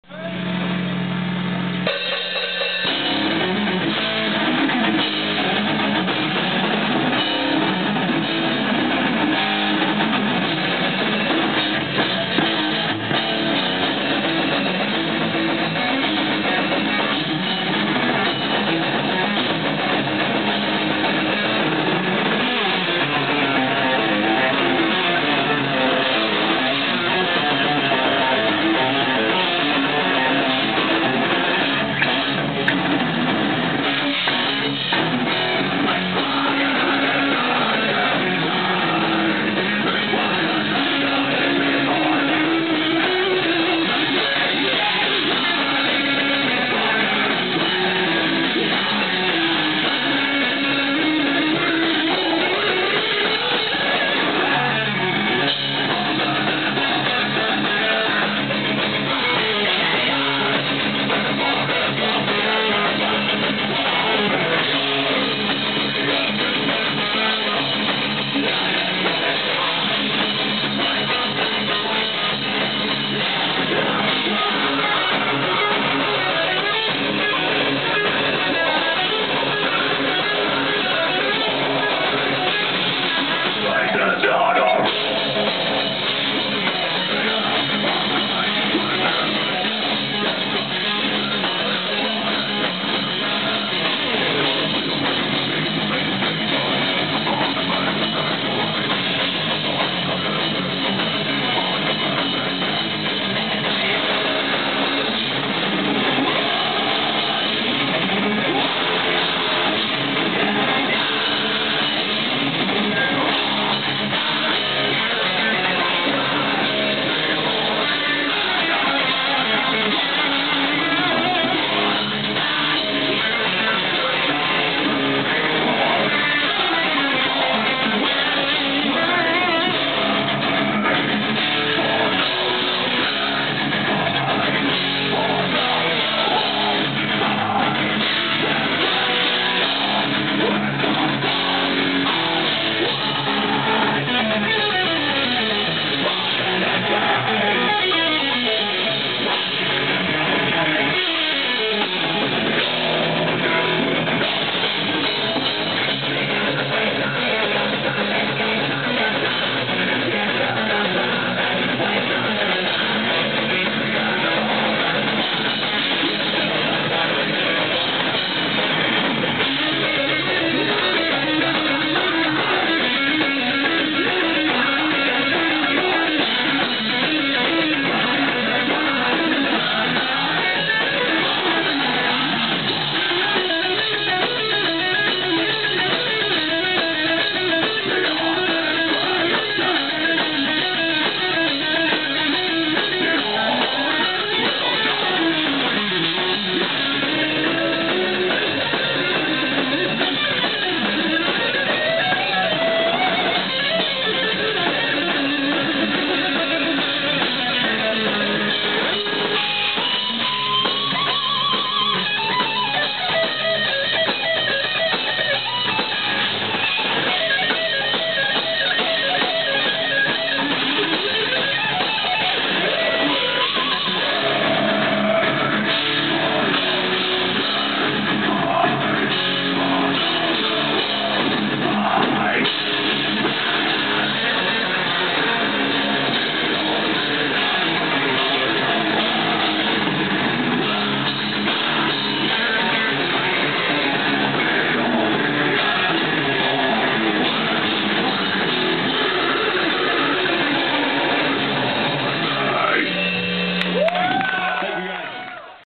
woe of tyrants - song 3 @ Kazemer
woe of tyrants playing live @ The Kazemer March 25 2008